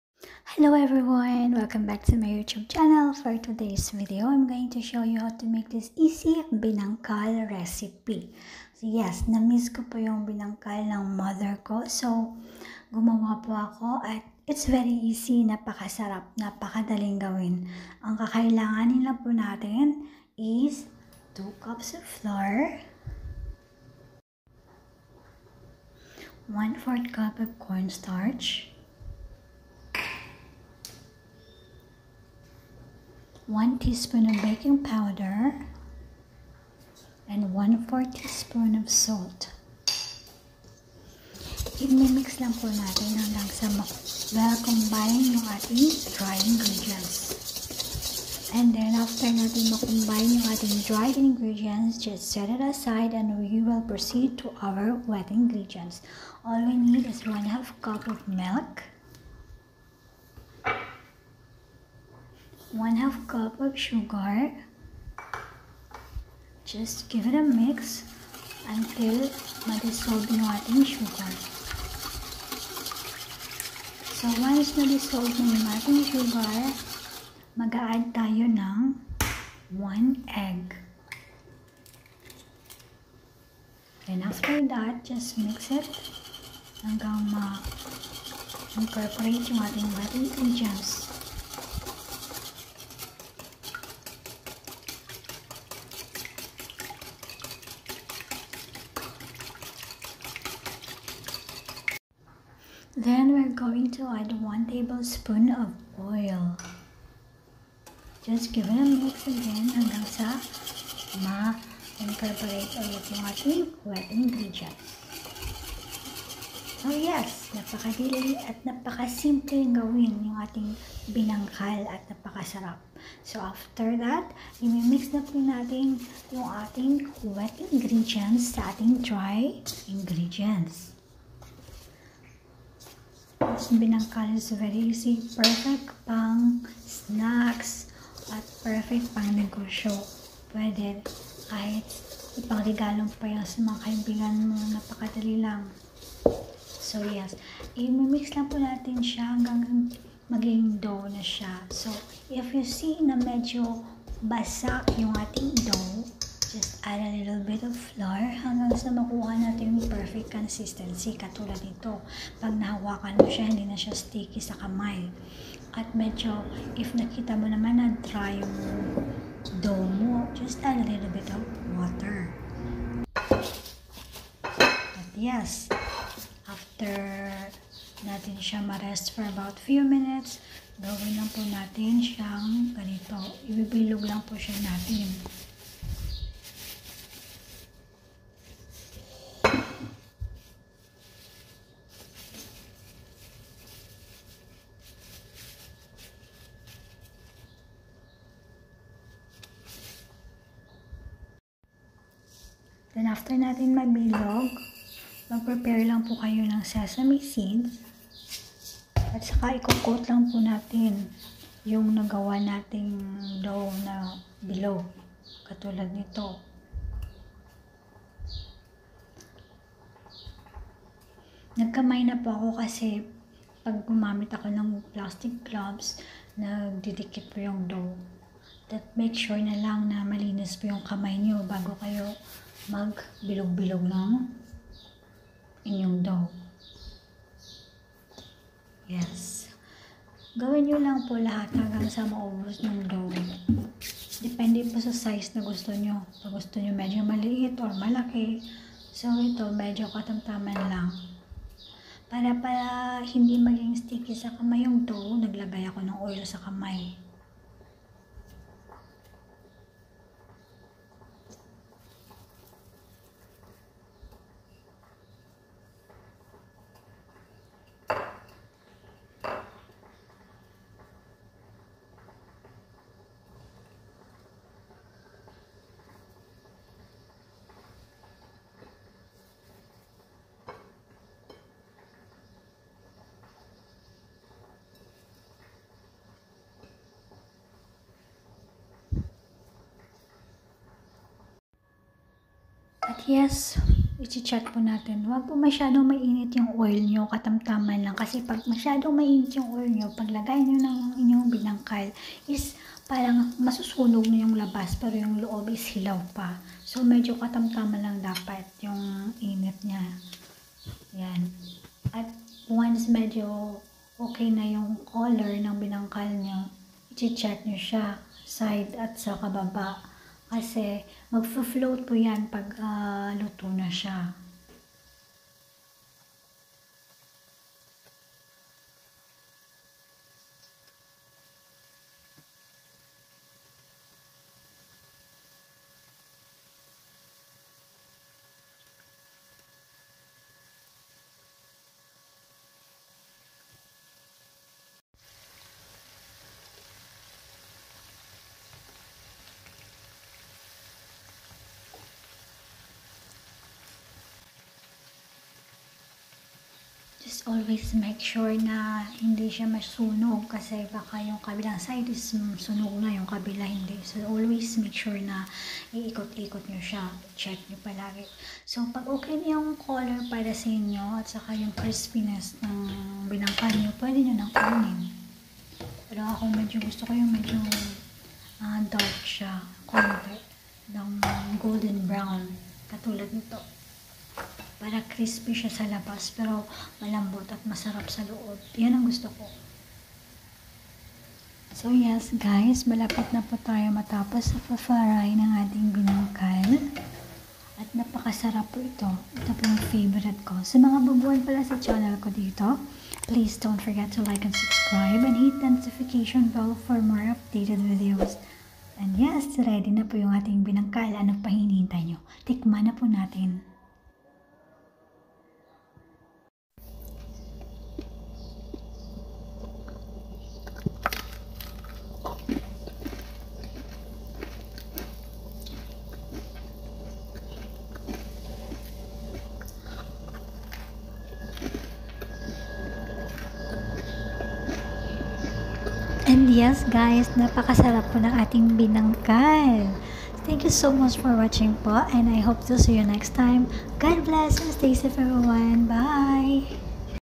Easy Binangkal Recipe
2cups flour
1/4 cup cornstarch
1tsp baking powder
1/4 tsp salt
1/2 cup milk
1/2 cup sugar
1 egg
1tbsp oil
sesame seeds for coating

Enjoy your Delicious Binangkal